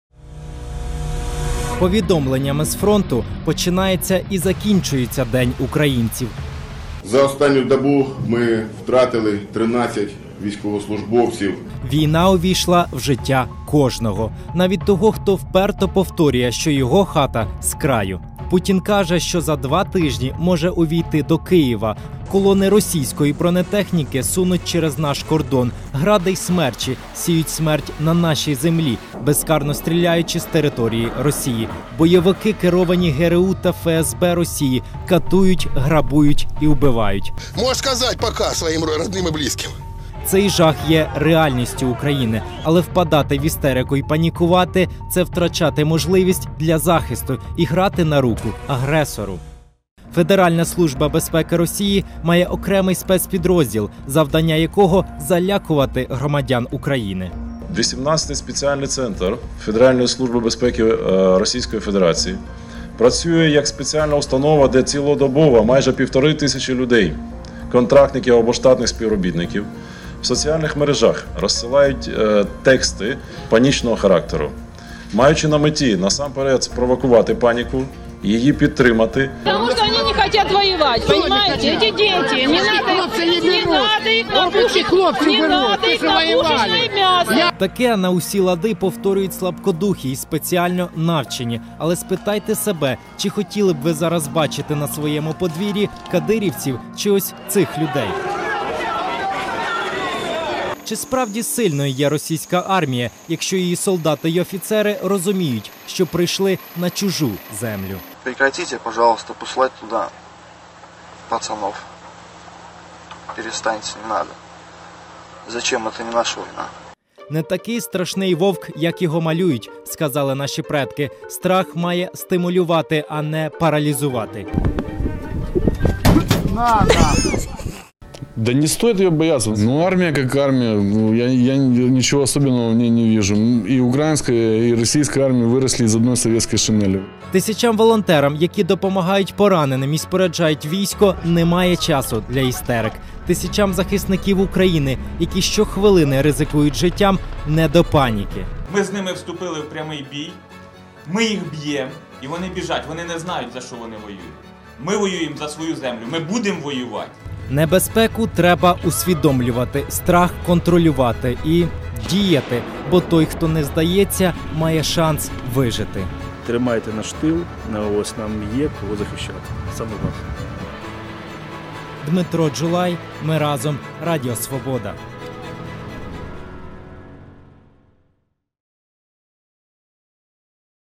Впадати в панікувати – втрачати можливості для захисту
Повідомлення з фронту починається і закінчується день українців. Війна увійшла в життя кожного. Навіть того, хто вперто повторює, що його хата скраю.